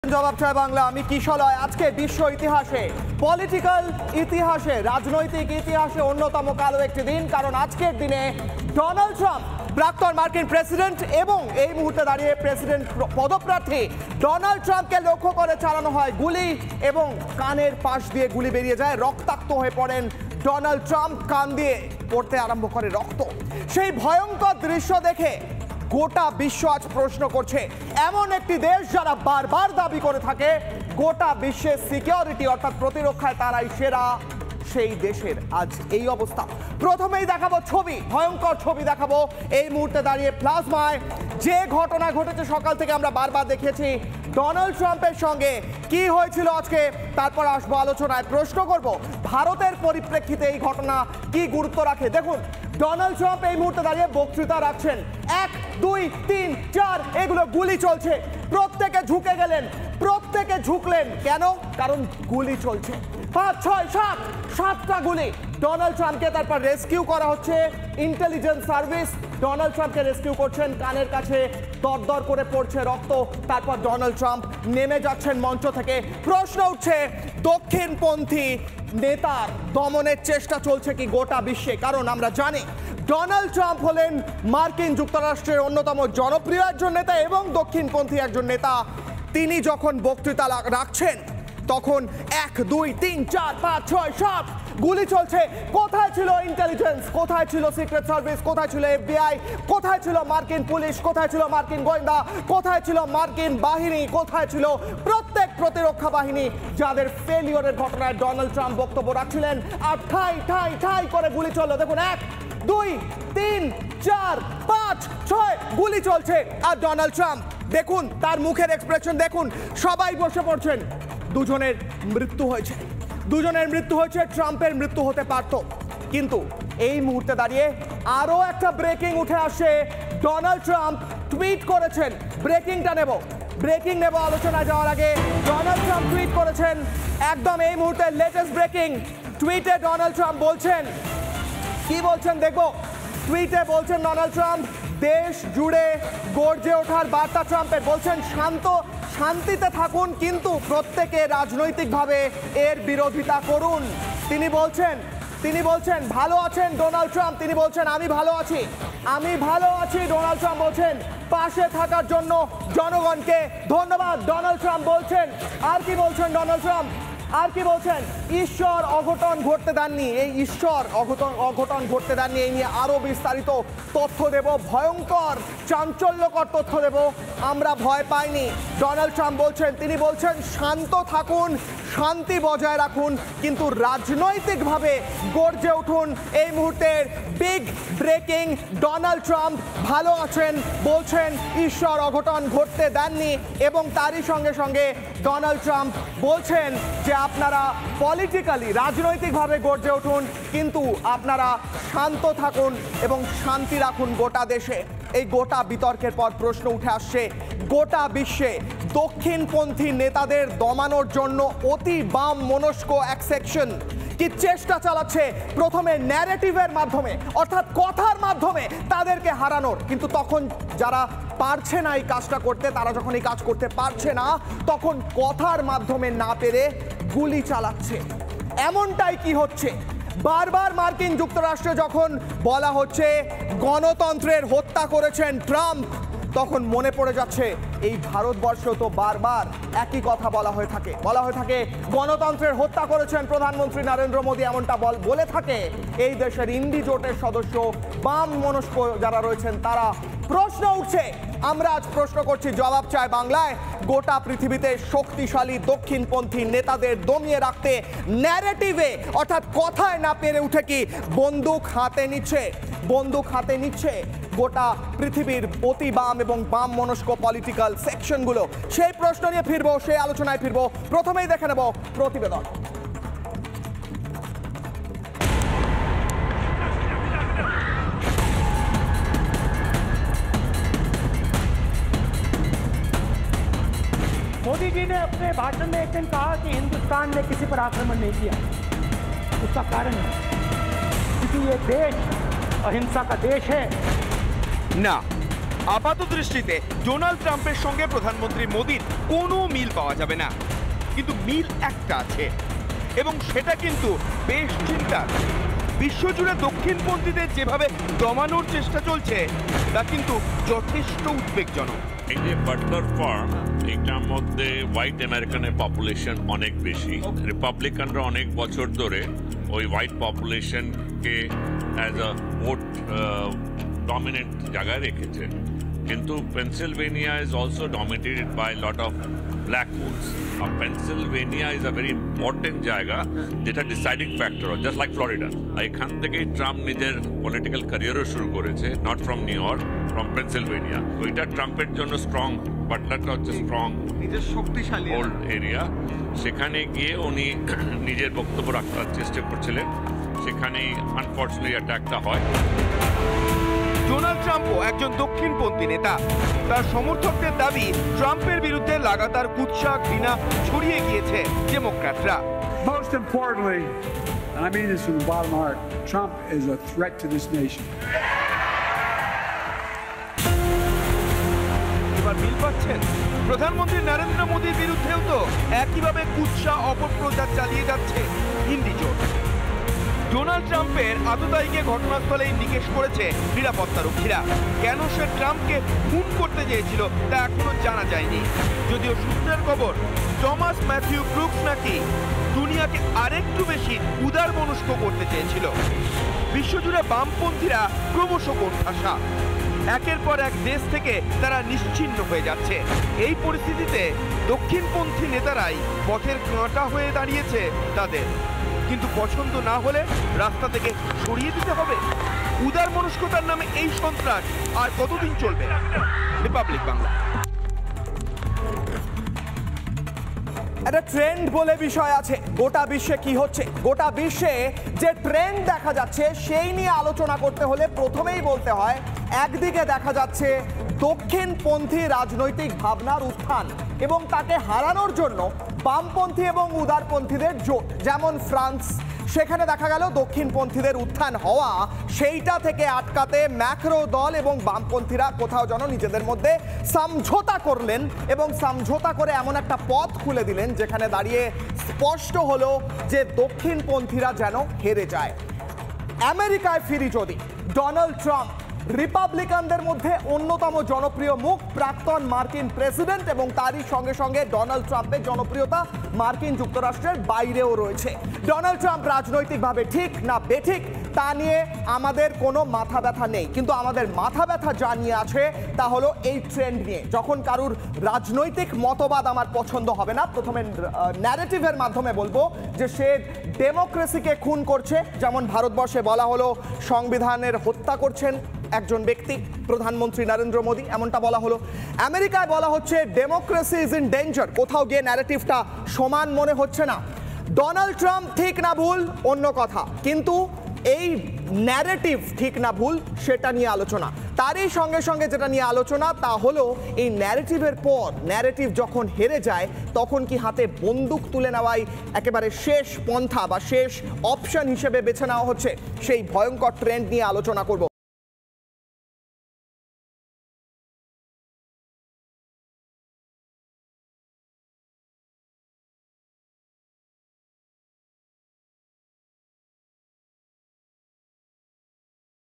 আততায়ীর গুলিতে জখম ট্রাম্প। বিশ্বজুড়ে দক্ষিণপন্থীদের দমানোর চেষ্টা।সরাসরি পথের কাঁটা সরানোর চেষ্টা?
আততায়ীর গুলিতে জখম ট্রাম্প। বিশ্বজুড়ে দক্ষিণপন্থীদের দমানোর চেষ্টা। হেয় করে, অত্যাচার করেও লাভ হচ্ছে না। সরাসরি পথের কাঁটা সরানোর চেষ্টা?

রিপাবলিক স্বাধীন।
রিপাবলিক নির্ভীক।
আপনিই রিপাবলিক।